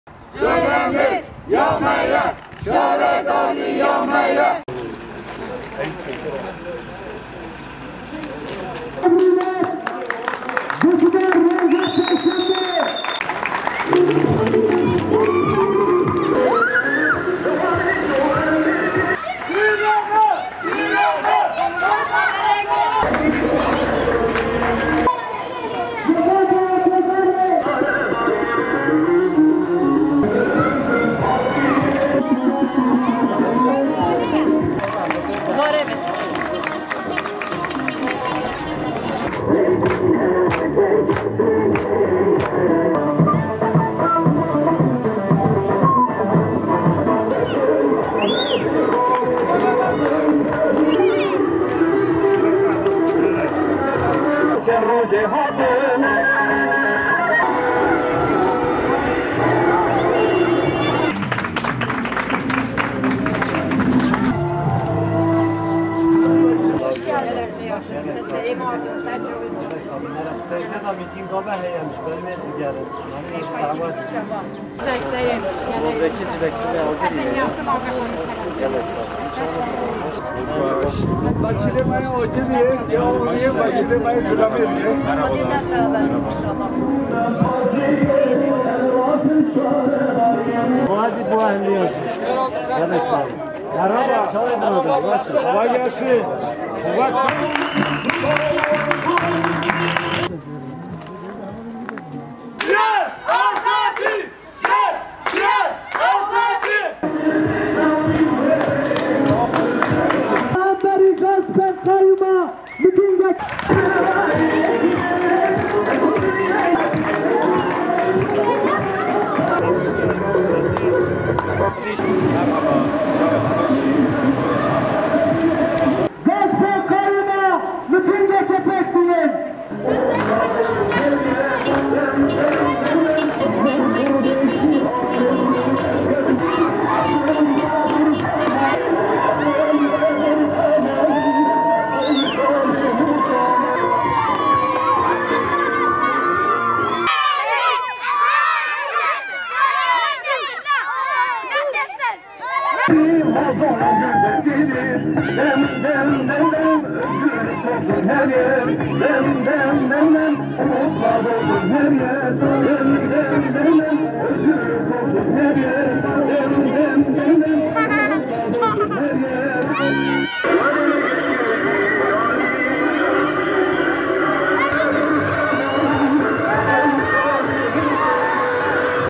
COLEMÊRG’DE GÜN BOYU MİTİNG ÇALIŞMASI
COLEMÊRG – Colemêrg'de irade gaspına karşı 13 Haziran'da yapılacak miting için tüm mahallelerde gün boyu çalışma yürütüldü.

Colemêrg Belediyesi'ne kayyım atanması kararına karşı kentte 13 Haziran’da “Belediyeler halkındır gaspa izin vermeyeceğiz” mitingi düzenlenecek. Halkların Eşitlik ve Demokrasi Partisi (DEM Parti) Colemêrg İl Örgütü öncülüğünde kente bağlı Pehlivan, Keklikpınar, Kıran, Biçer, Berçelan, Tekser, Gazi, Merzan, Yeni Mahalle, Bağlar, Karşıyaka, Sümbül, Medrese ve Bulak mahallelerinde miting çalışması yürütüldü.